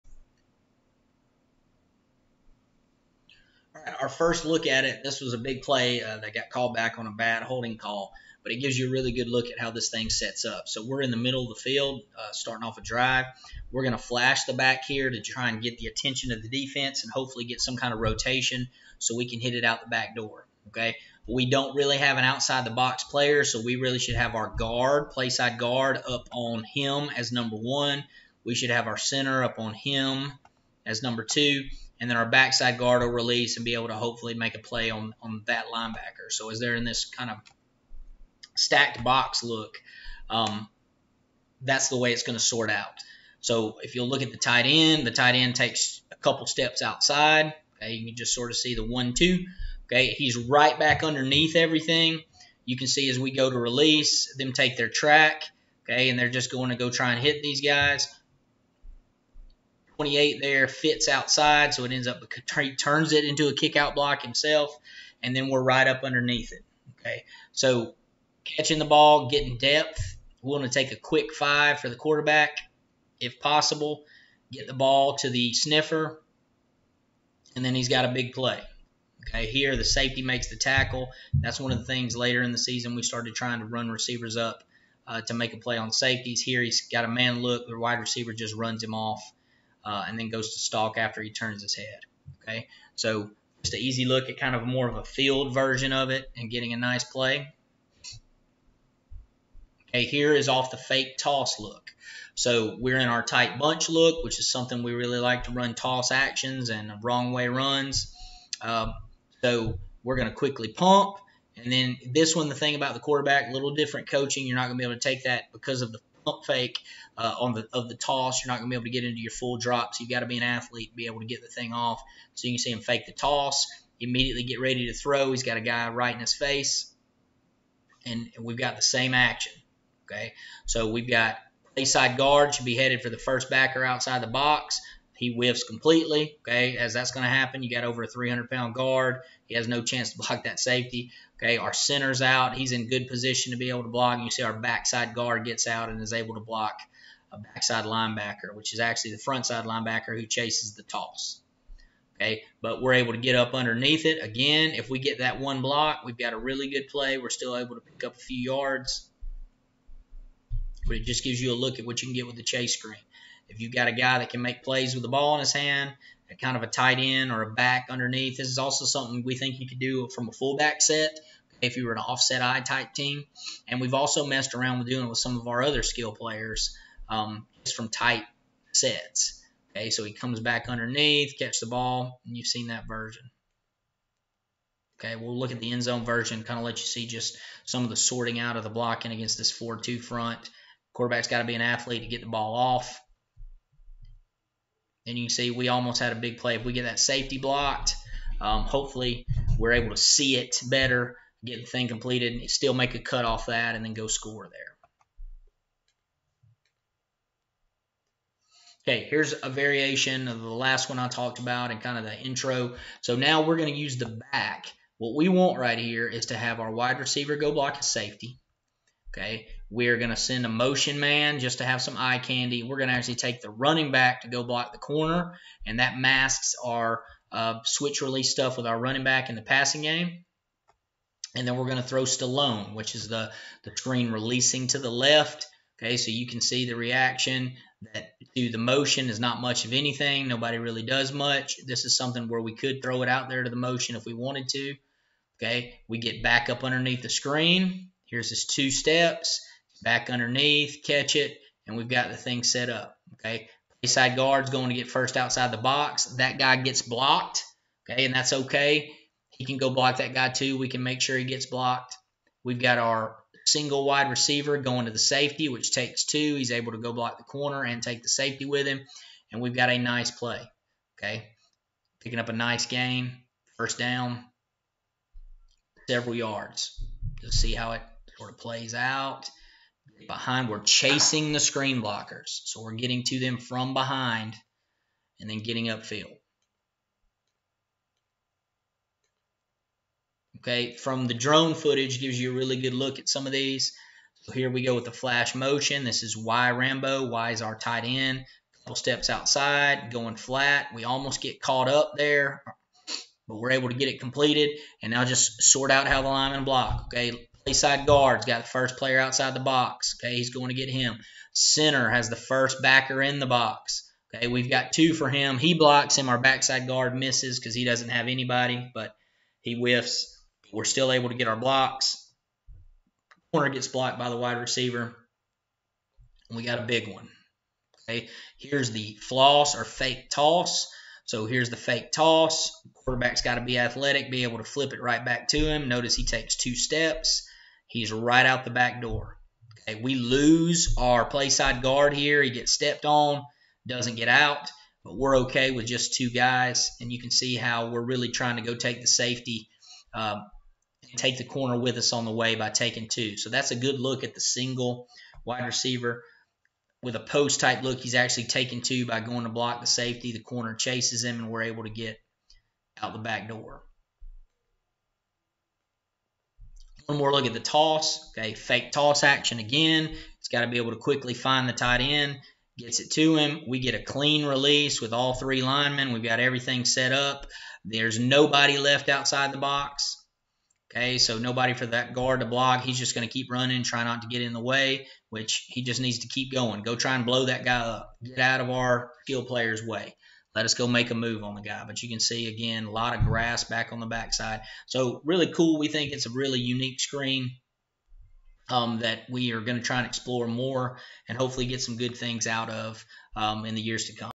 Jones County (GA) OC Josh Herring - Chase Screen
"Chase Screen" from Jones County (GA) OC Josh Herring. Great constraint play & way to feature a dynamic TE if you have one. Coach Herring has produced the leading passer in Georgia football in both 2019 & 2021.